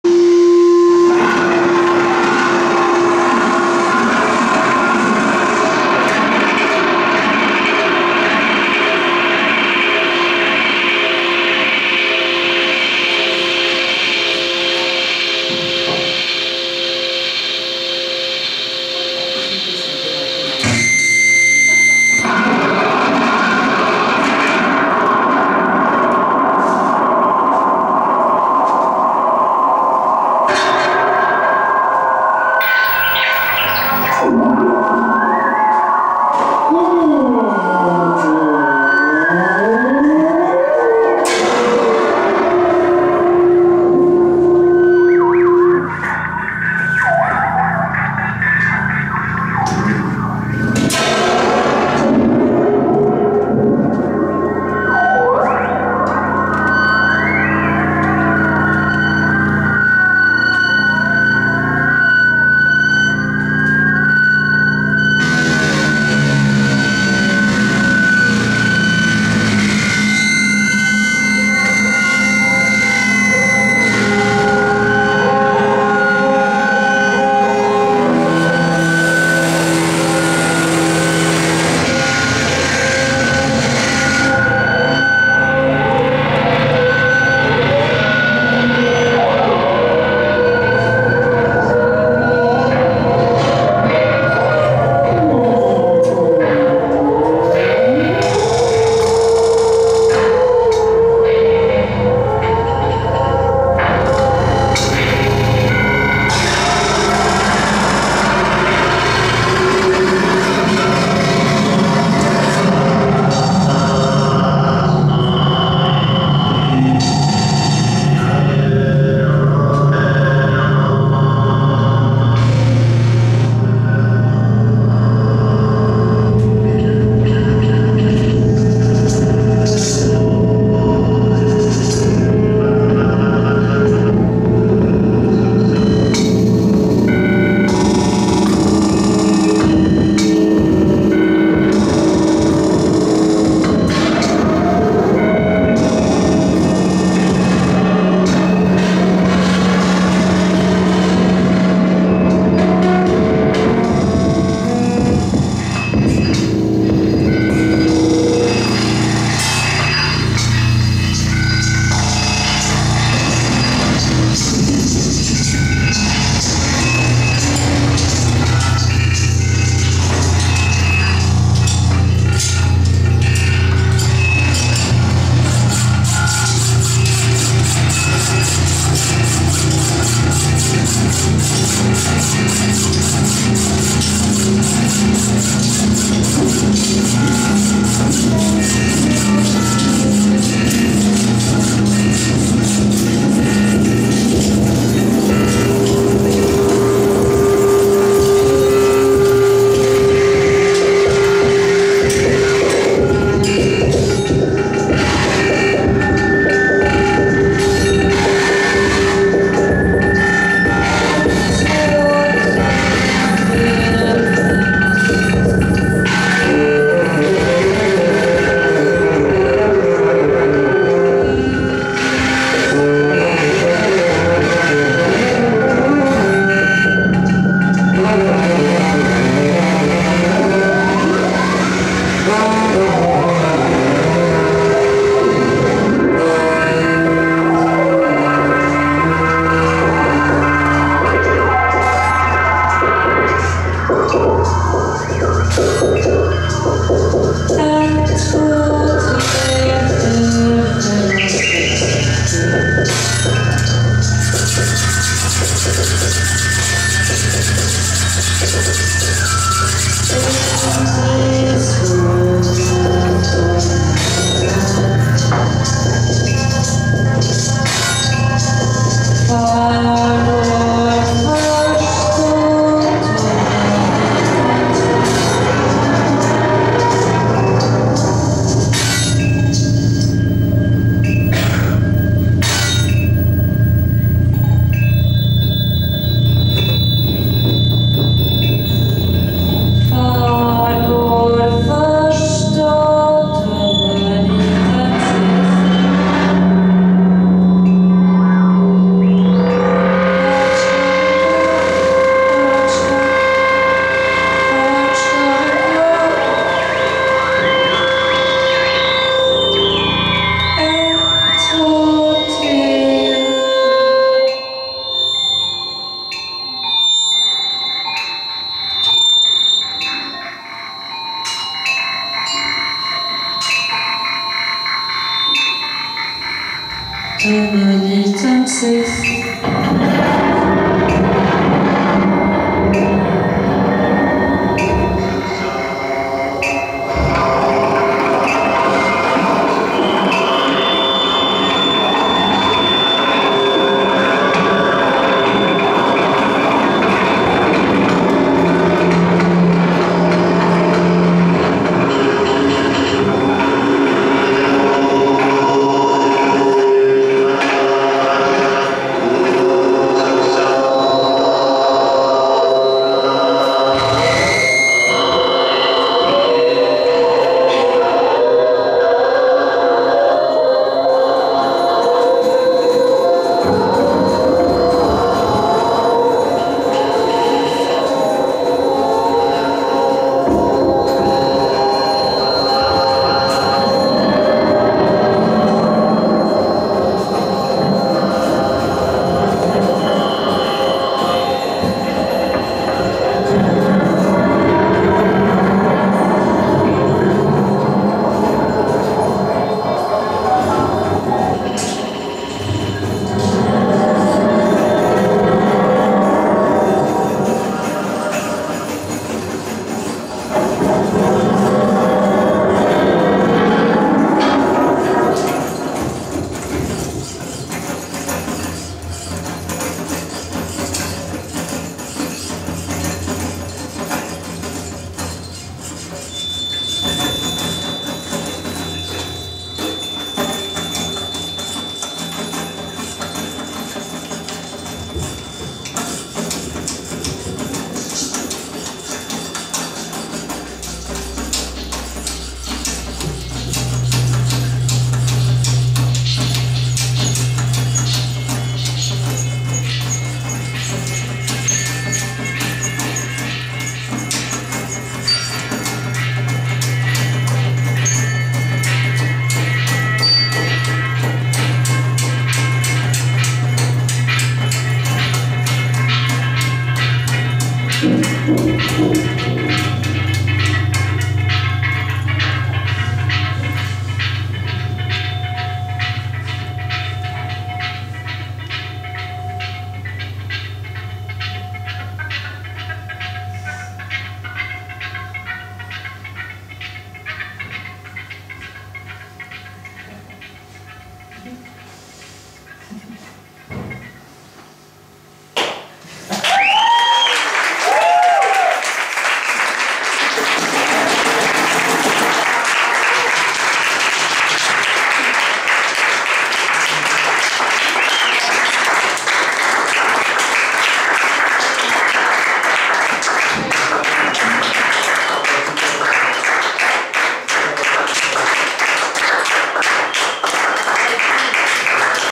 Guido Flichman : Barbara Togander : Javier Bustos : Luis Conde : Juan De Cicco : Facundo Rivarola ::
Festival del Microclima
una.casa - dic. 17 2016 -
8 músicos / 8 sets librados al azar (bolillero de por medio)